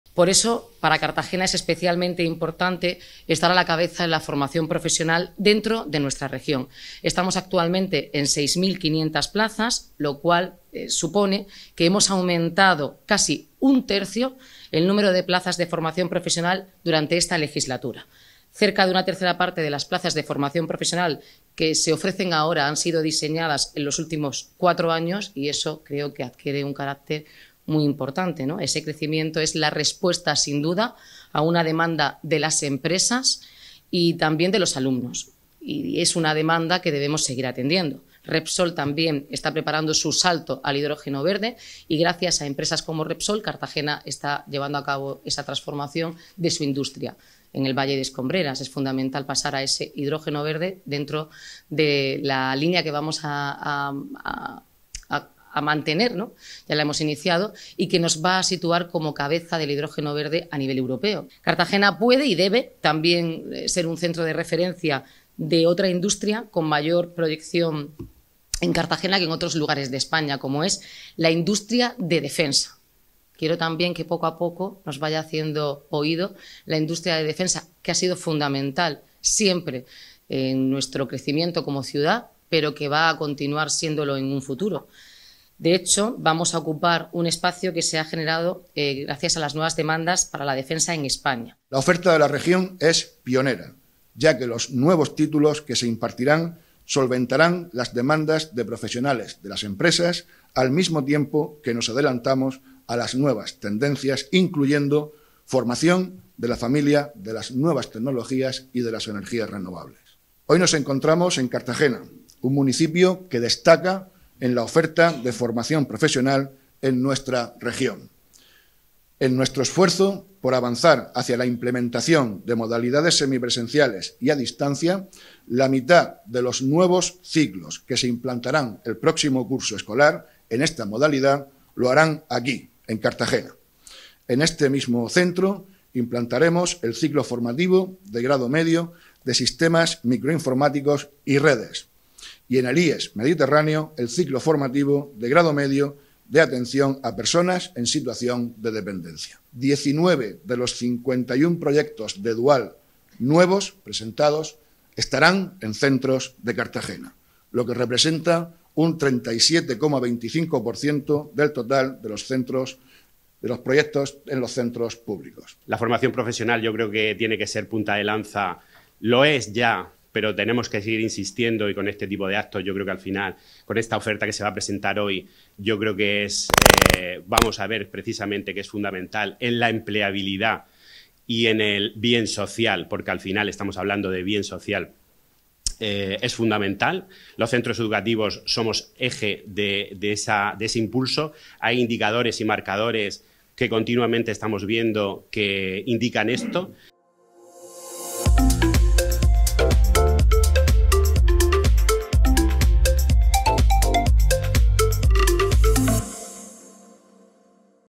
Cartagena encabeza la oferta de Formación Profesional dual dentro de la Región de Murcia
La Formación Profesional es un pilar fundamental en la empleabilidad y el bien social de la sociedad. Se ha presentado, en el CIFP Carlos III, la oferta formativa de FP de la Región de Murcia para el curso 2023/2024 con 39.000 plazas, nuevos ciclos formativos y cursos de especialización adecuados a la demanda empresarial con el fin de incrementar la empleabilidad del alumnado. Destaca el crecimiento de la FP dual en Cartagena, encabezando la oferta de la Región de Murcia, con hasta 6.500 plazas.

Han presentado la nueva oferta Noelia Arroyo, junto al consejero de Educación, Formación Profesional y Empleo, Víctor Marín y el director del CIFP Carlos III, Francisco Hernández. También ha estado presente Irene Ruíz, concejal del área de Educación.

En total se ofrecerán 167 títulos de Grado Básico, Medio, Superior y Cursos de Especialización, más siete certificados que se obtienen a través de los correspondientes Programas Formativos Profesionales, distribuidos en 750 ciclos formativos, con una inversión prevista de 186 millones de euros, ha señalado Víctor Marín.

Durante el acto, Arroyo ha señalado que el crecimiento de las plazas de formación profesional que se ofertan en Cartagena responde a una demanda de las empresas y de los alumnos “que debemos seguir atendiendo”.

La alcaldesa ha destacado el crecimiento de la oferta en plazas, y también de enseñanza: la oferta de ciclos de FP ha crecido desde 2019 un 27%, pero ese aumento ha sido especialmente importante en los ciclos de grado superior. Actualmente hay 54, y uno de cada tres ciclos de grado superior de los que se impartan este año serán de nueva creación.

Marín ha puesto como ejemplo a Cartagena “que cuenta con un gran número de ciclos formativos relacionados con el sector naval y químico, punteros en la comarca”. Para el próximo curso se implantará el Grado Medio de Sistemas microinformáticos y redes en el CIFP Carlos III, y en el IES Mediterráneo, el Grado Medio de Atención a Personas en situación de dependencia.

Además, se realizarán obras de ampliación del CIFP de Hostelería y Turismo, se ampliará el IES Las Salinas de La Manga con los ciclos de Cocina y Restauración, se adecuará el antiguo colegio Antonio Arévalo para albergar más enseñanzas de FP y se está tramitando el proyecto de sustitución del Centro Integrado de FP Hespérides.

“Debemos seguir creciendo en oferta, especialmente en formación dual para que nuestras empresas tengan los mejores trabajadores en los procesos de transformación tecnológica que están afrontando en este momento”, ha señalado Arroyo.

La FP Dual ha aumentado en Cartagena casi en un tercio el número de plazas, en los últimos cuatro años, hasta las 6.500.Este hecho “es especialmente importante para que Cartagena pueda estar en cabeza en la formación profesional dentro de la Región, ya que, una formación profesional bien diseñada es una puerta directa al empleo”, ha dicho la alcaldesa, que sitúa los niveles de empleabilidad en torno al 60% en FP y en el 100% en la FP Dual, “entre los mayores de España”.

Además, la regidora se se ha referido concretamente a la empresa Repsol, que participa en programas duales y está aprovechando la oportunidad de formar a sus alumnos en plena transformación hacia el uso de biocombustibles. “Están formando al personal del futuro mientras construyen la empresa del futuro”, ha señalado Arroyo.

La alcaldesa se ha referido a la oportunidad de futuro que supone el hidrógeno verde, y reclama ser un centro de referencia en la formación para trabajadores en hidrógeno verde formados en los centros de FP de Cartagena mientras avanza esta tecnología.

También ha asegurado que Cartagena puede y debe ser un centro de referencia en la formación de la otra industria con mayor proyección en la ciudad, la industria de la Defensa.

También ha hecho mención al ranking de ciudades que más invierten en educación en España, del que Cartagena ocupa la sexta posición entre los ayuntamientos de más de 200.000 habitantes, que recoge inversiones en educación en forma de obras en colegios o en ayudas a comedores, aunque ha destacado otros esfuerzos en formación como los que realiza ADLE.  Arroyo ha afirmado, “que este año hemos multiplicado por 2. Este año tenemos 3,2 millones de euros para estos programas de formación, como la FP, orientada al empleo y diseñada junto a las empresas”.